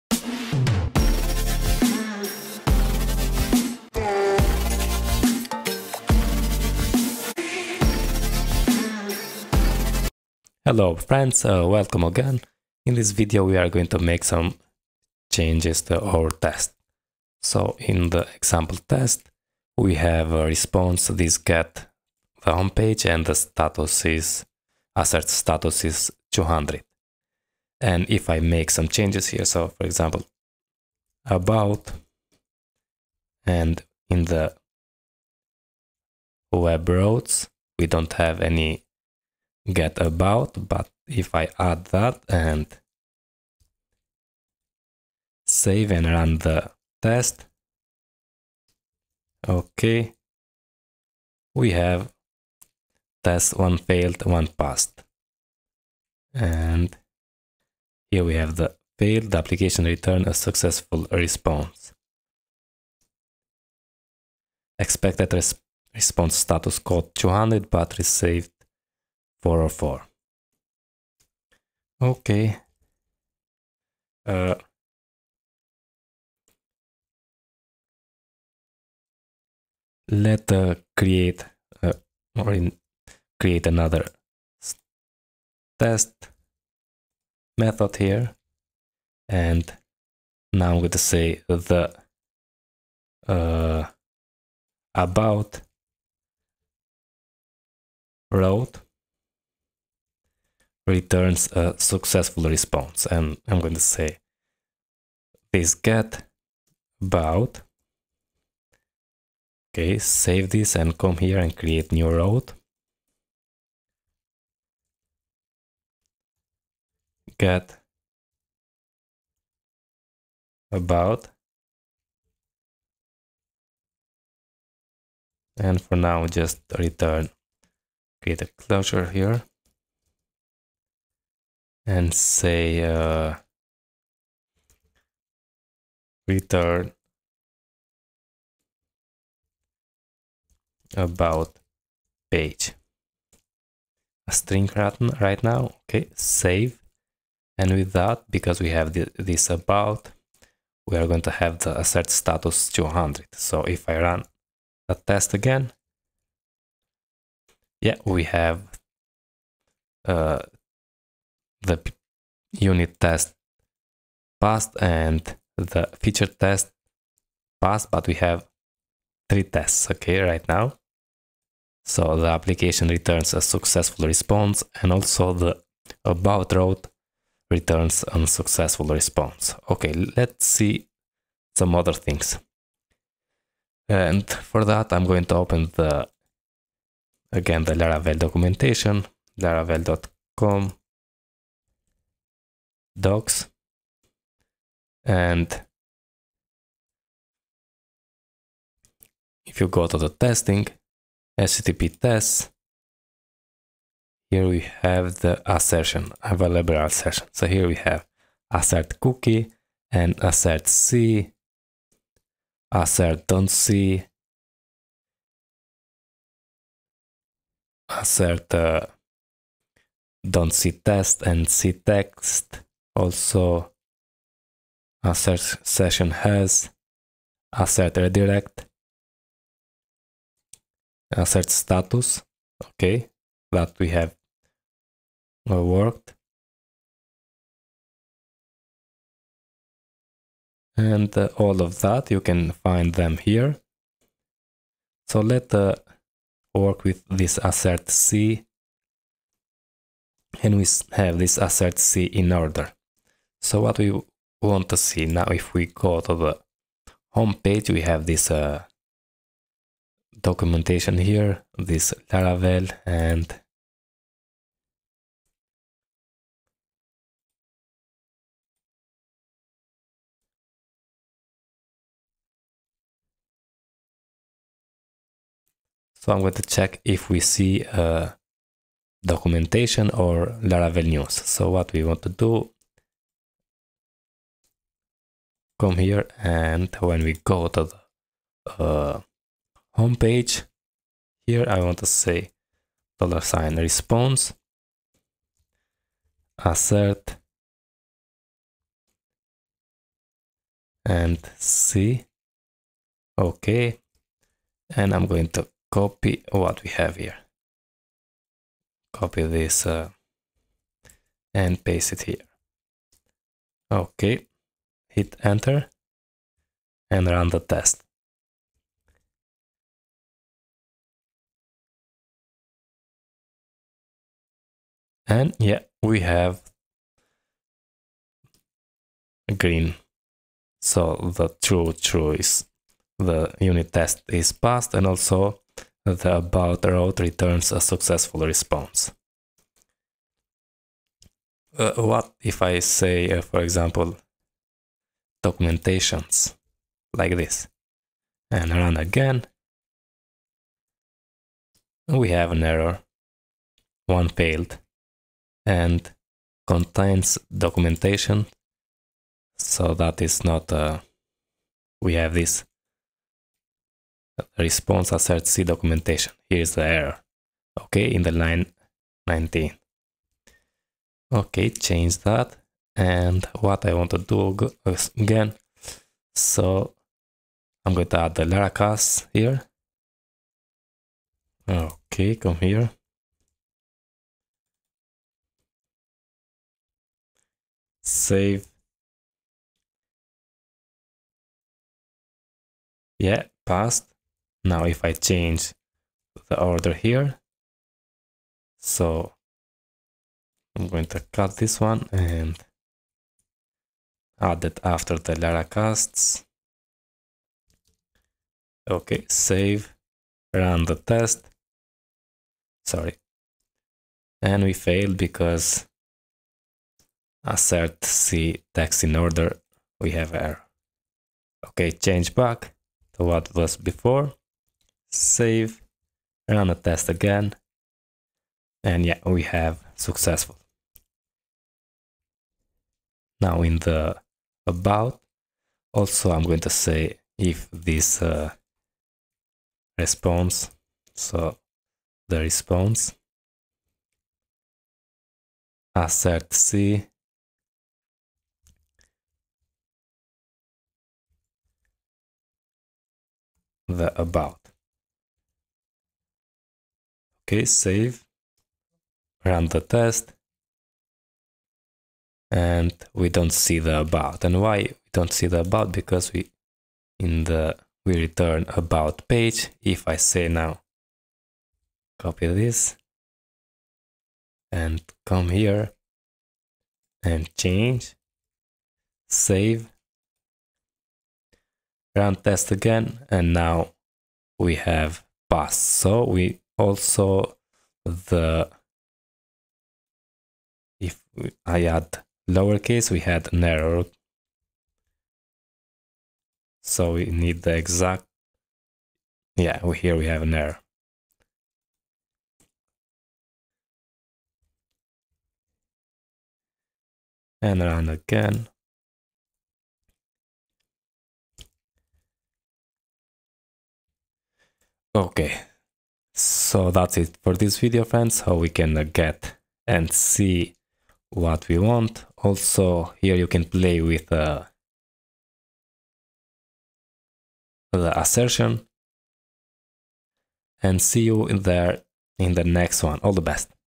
#3 Testing in Laravel | Make Your First Tests | Laravel 9 Testing Tutorial
This video will show you how to make your first tests in Laravel. We are going to cover the basics of unit tests, feature tests, HTTP tests, database tests in Laravel.